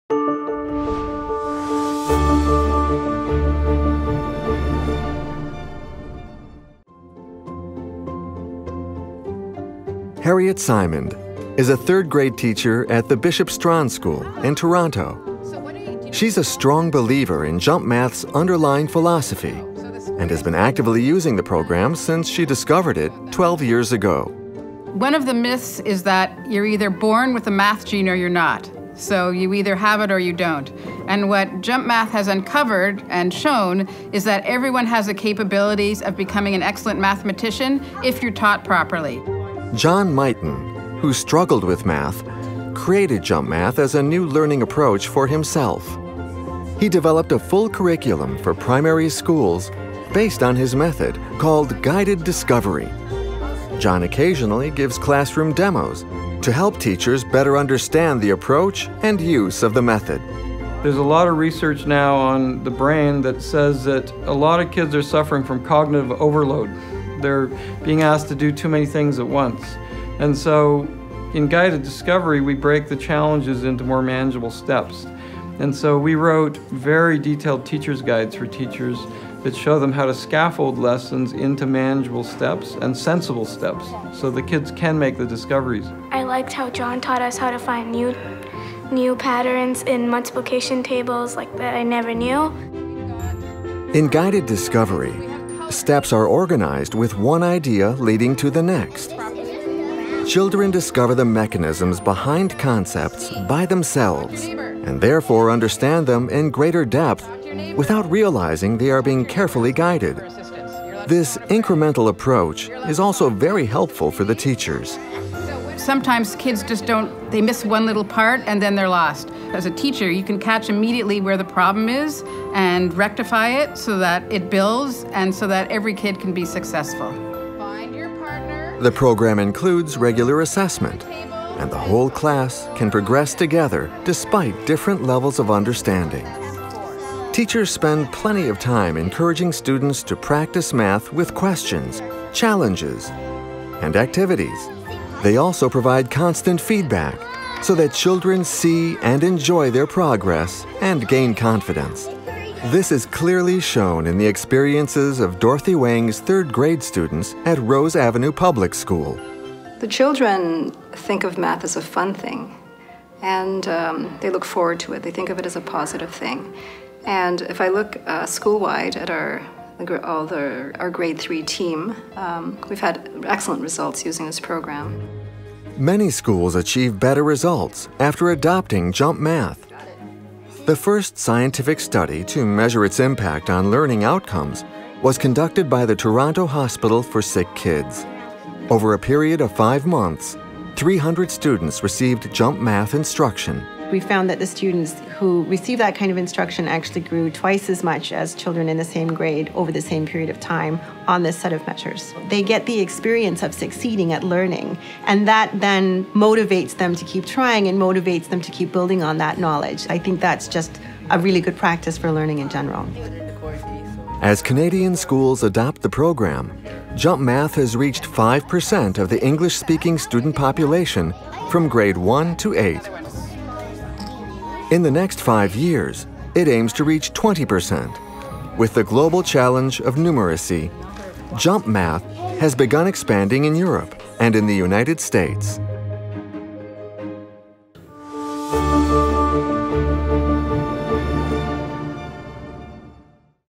2016 WISE Award Winner: JUMP Math, Canada
JUMP Math uses a guided discovery method to teach K to 8 math in Canada, the USA, Spain, and Bulgaria. The program provides comprehensive, mostly free resources for teachers. Impact studies show that the program has significantly increased learning outcomes in comparison to conventional math programs.